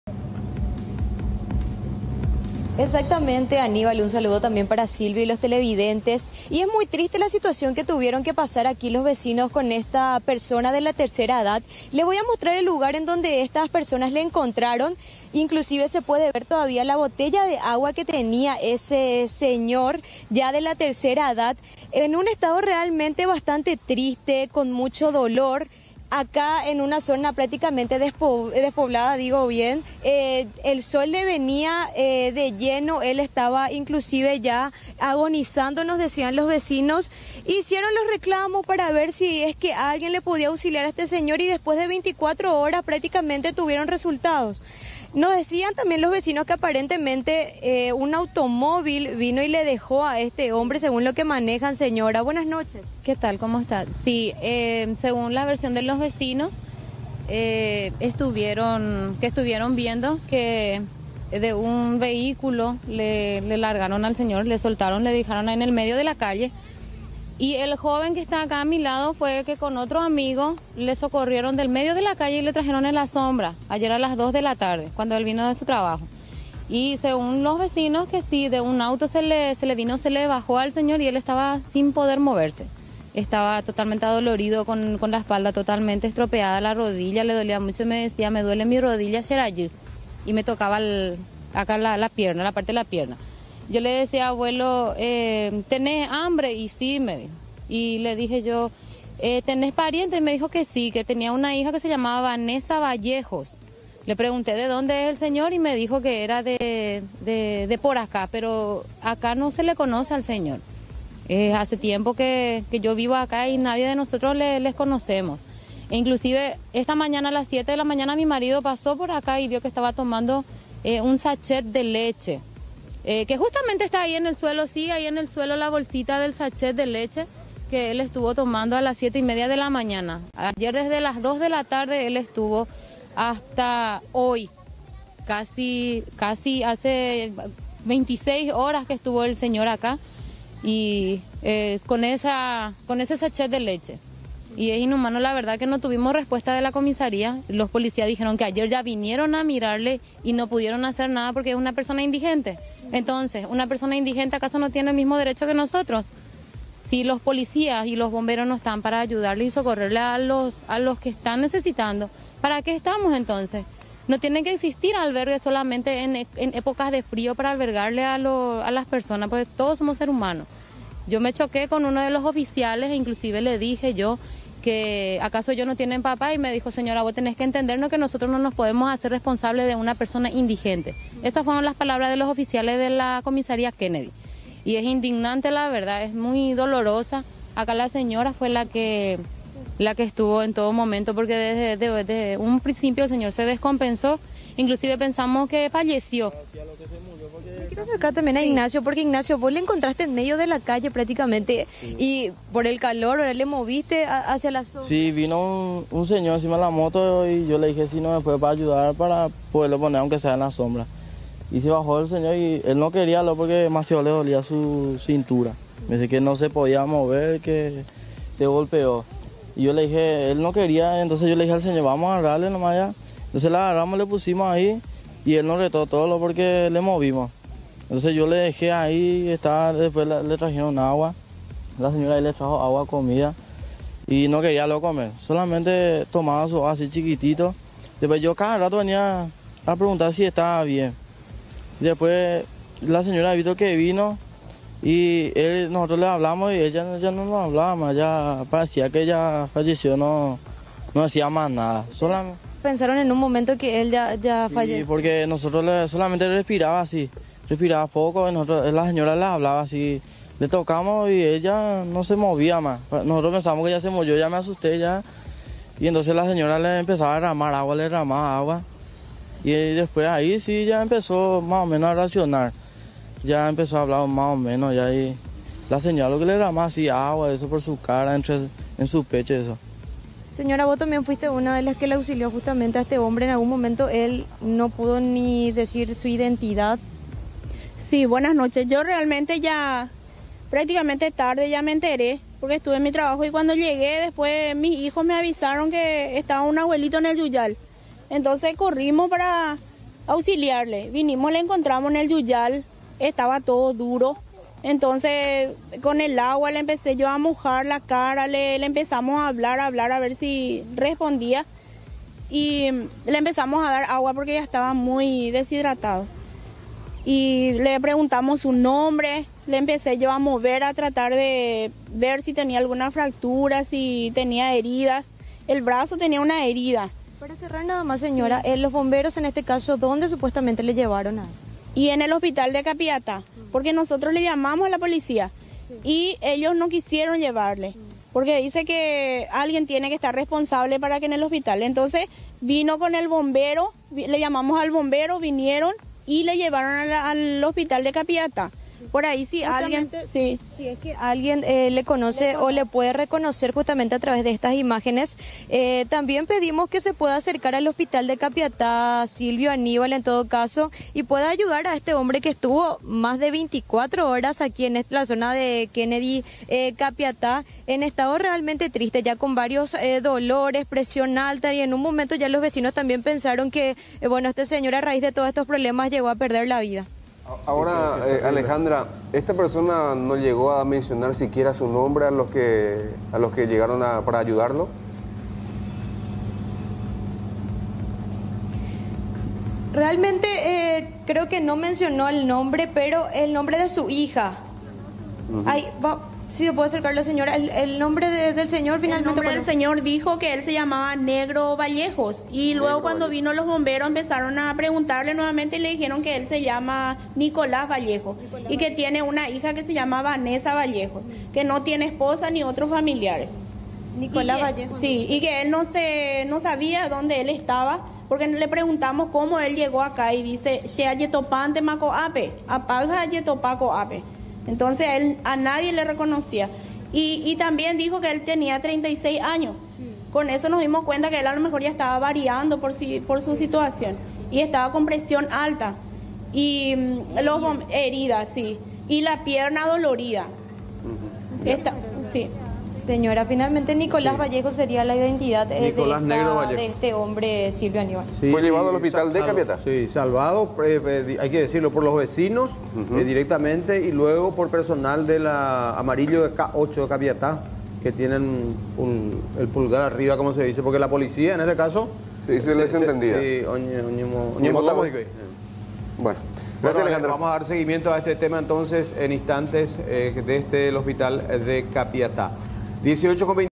Sexagenario fue abandonado por sus familiares
Vecinos alertaron a las autoridades. El hombre fue abandonado con una botella de agua, el hombre se encontraba agonizando en la intemperie.